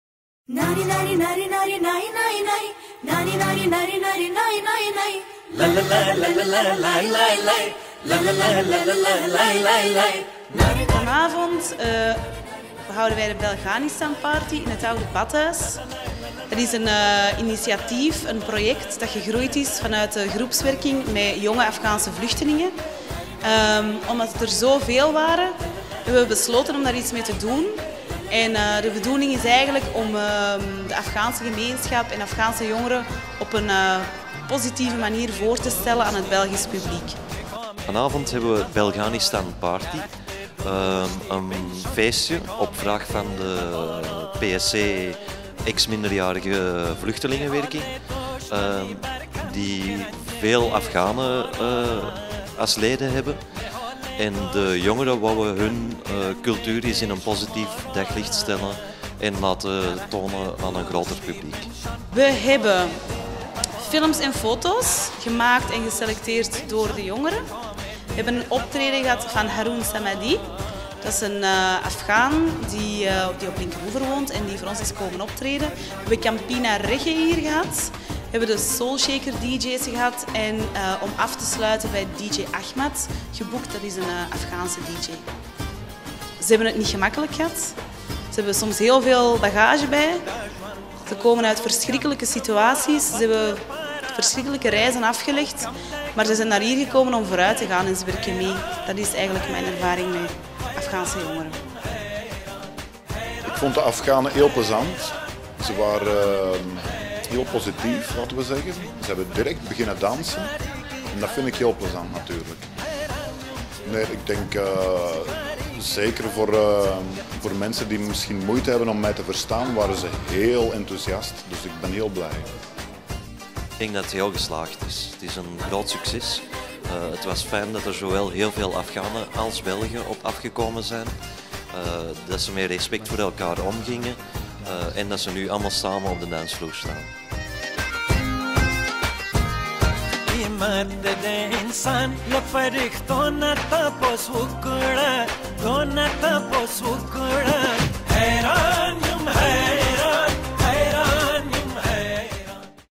Belghanistan party doet Oude Badhuis uit voegen barsten
Op 15 oktober kon je in het Oude Badhuis kennismaken met het land van vliegeraars, paardrijders en meeslepende muzikale hoogtepunten. Geen mannen met baarden dus, maar enkele honderden jonge Afghaanse vluchtelingen op de dansvloer.
Het Protestants Sociaal Centrum (PSC) nodigde iedereen uit om samen met het hippe Afghaanse volkje kennis te maken met de Afghaanse cultuur via live muziek, een fototentoonstelling en videoprojecties.

Muzikale omkadering werd verzorgd door Campina Reggae, Dosti, DJ SoulShaker en DJ Ahmad.